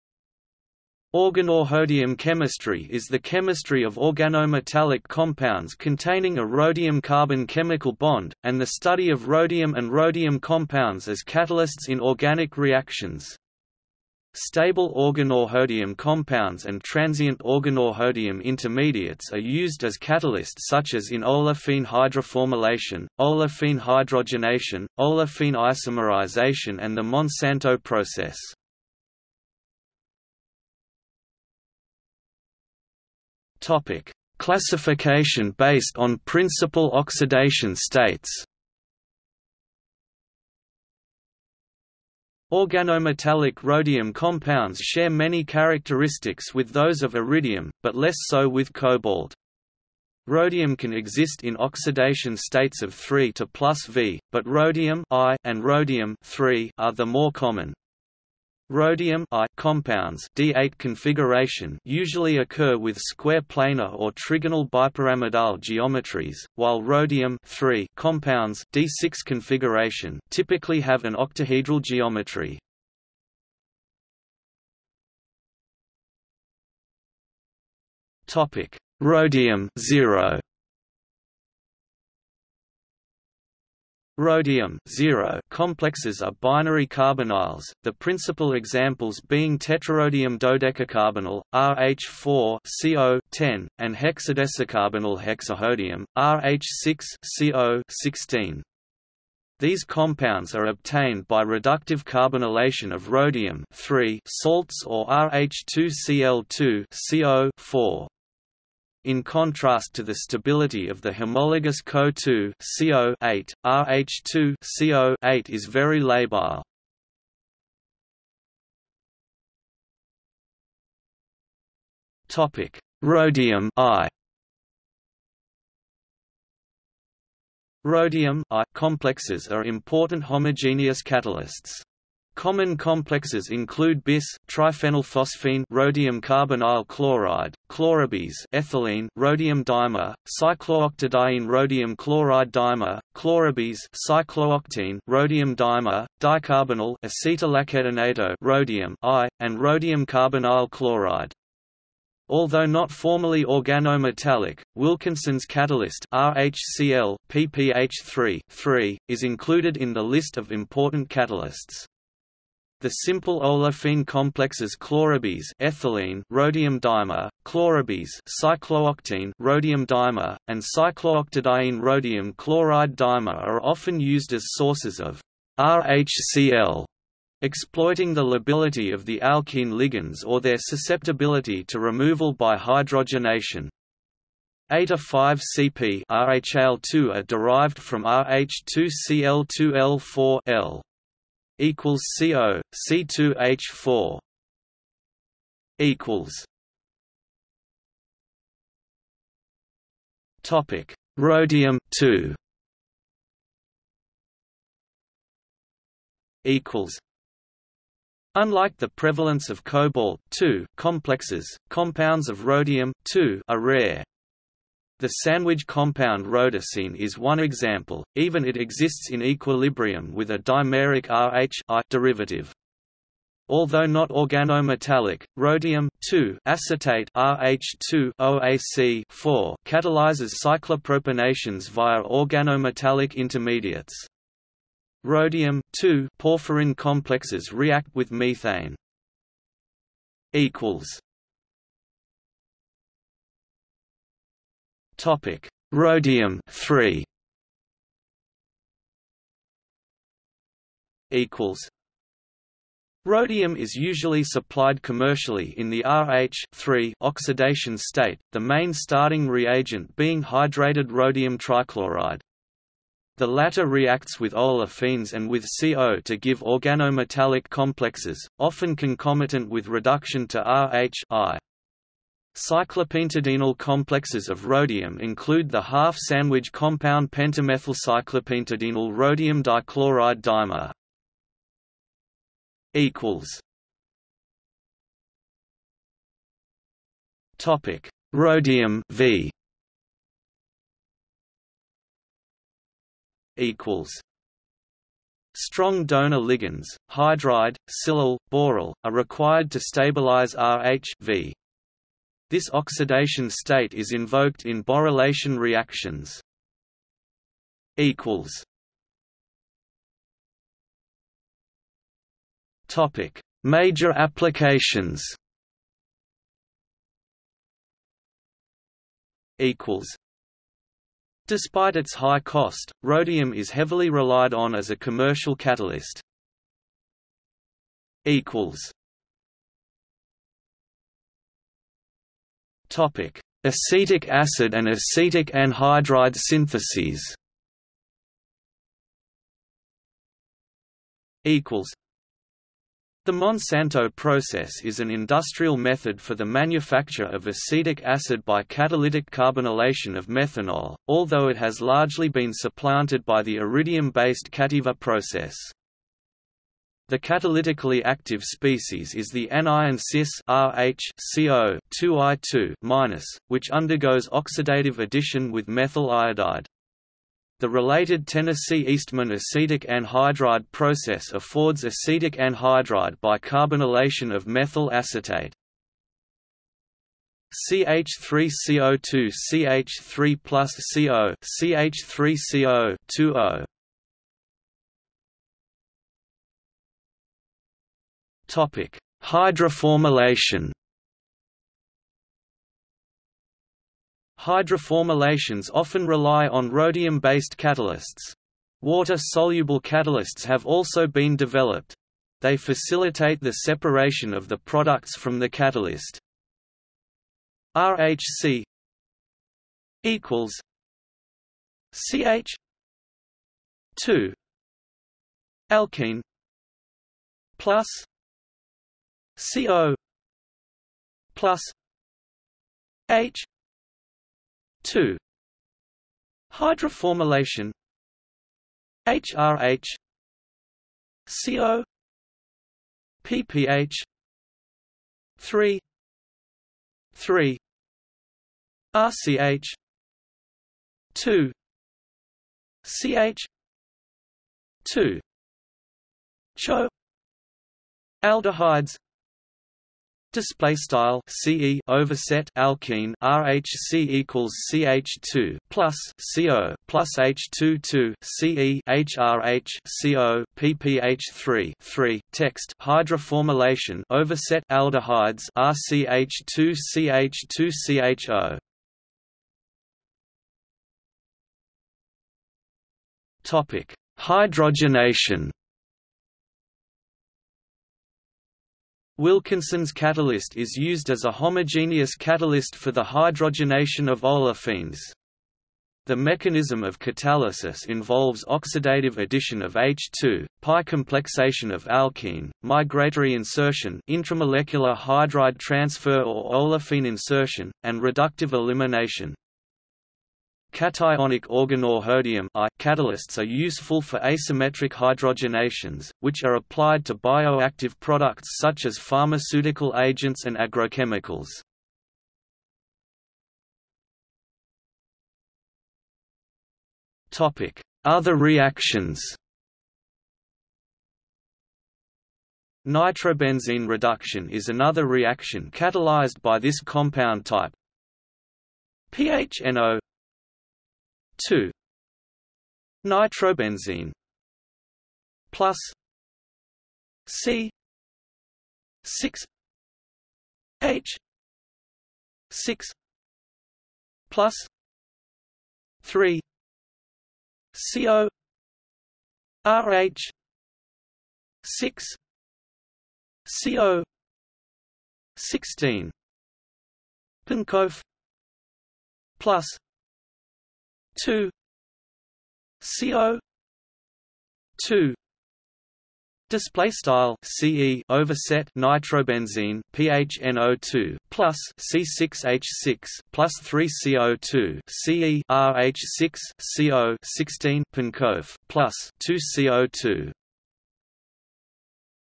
Organorhodium chemistry | Wikipedia audio article
00:00:29 1 Classification based on principal oxidation states
00:01:14 1.1 Rhodium(0)
00:01:58 1.2 Rhodium(I)
00:03:11 1.3 Rhodium(II)
00:03:16 1.4 Rhodium(III)
00:03:26 1.5 Rhodium(V)
00:04:01 2 Major applications
00:04:11 2.1 Acetic acid and acetic anhydride syntheses
00:04:43 2.2 Hydroformylation
00:04:53 2.3 Hydrogenation
00:05:08 2.4 Other reactions
00:05:19 3 References
00:05:28 Acetic acid and acetic anhydride syntheses
00:06:24 Hydroformylation
00:07:49 Hydrogenation
00:08:33 Other reactions

- Socrates

SUMMARY
Organorhodium chemistry is the chemistry of organometallic compounds containing a rhodium-carbon chemical bond, and the study of rhodium and rhodium compounds as catalysts in organic reactions.Stable organorhodium compounds and transient organorhodium intermediates are used as catalyst such as in olefin hydroformylation, olefin hydrogenation, olefin isomerization and the Monsanto process